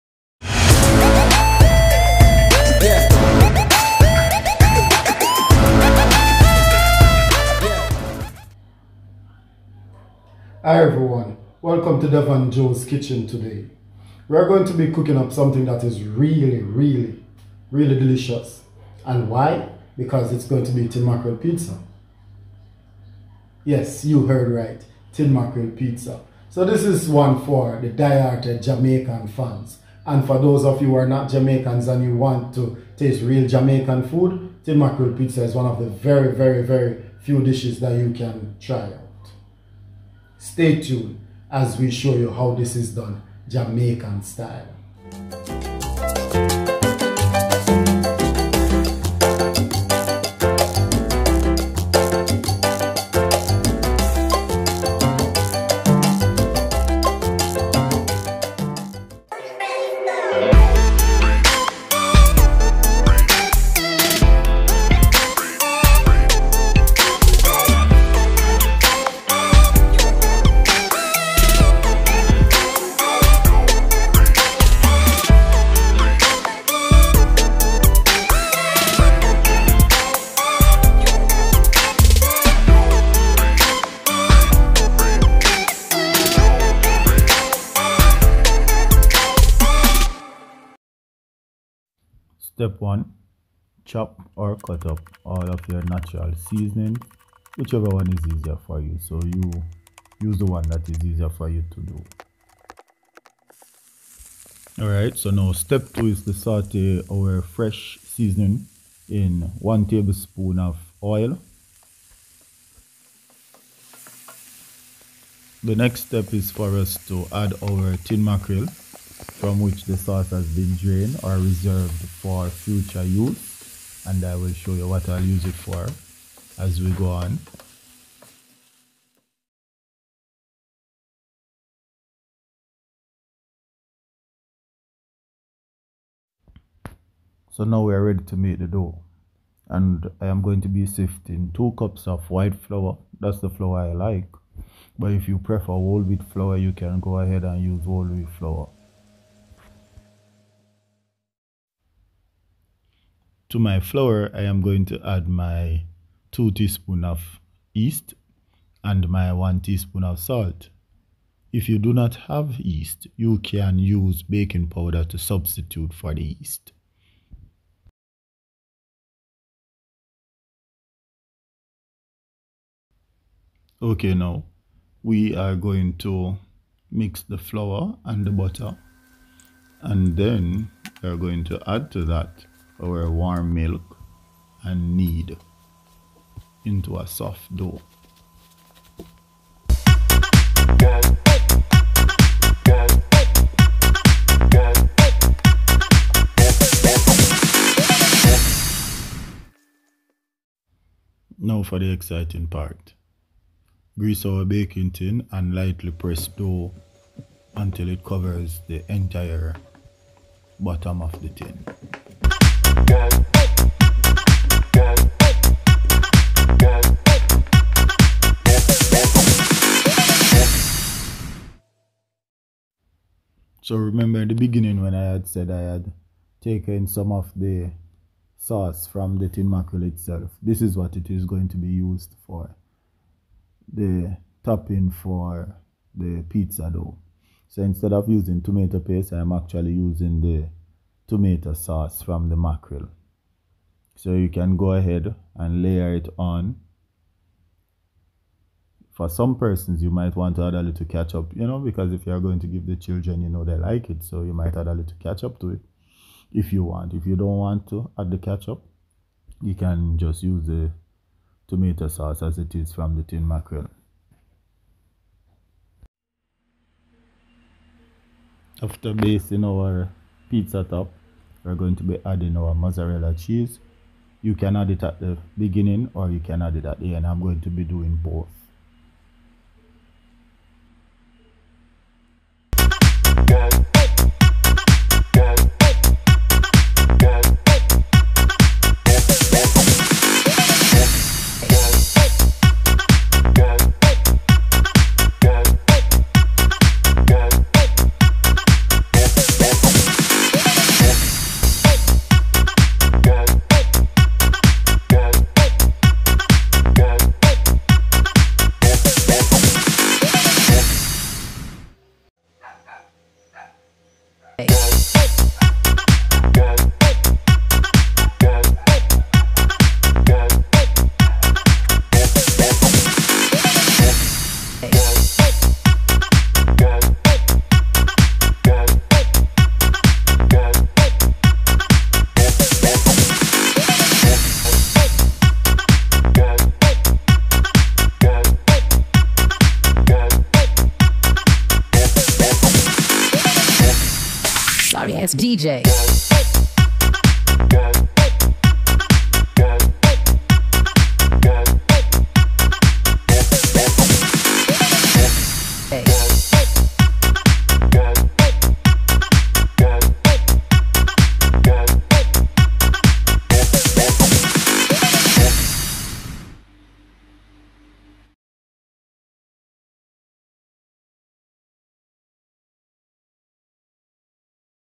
Tin Mackerel Pizza
This idea came to me many years ago when my father died and everyone was home. It felt good having everyone there but sad because one of my main motivators was missing.

My siblings who thought I was a good chef asked me to make pizzas and guess what was one of the three that we made?

This pizza brings back sad but joyous memories- when you try it you will only have fun memories.

Go ahead try it and then leave a comment after.